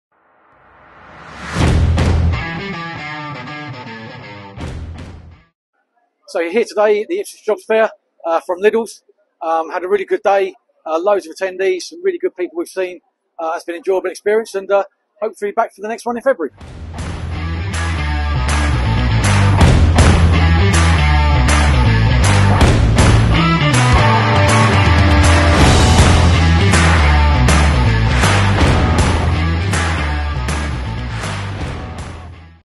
Lidl at the Ipswich Jobs Fair on Wednesday 20th September 2023.
Exhibitor Feedback from Lidl at the Ipswich Jobs Fair at Ipswich Corn Exchange.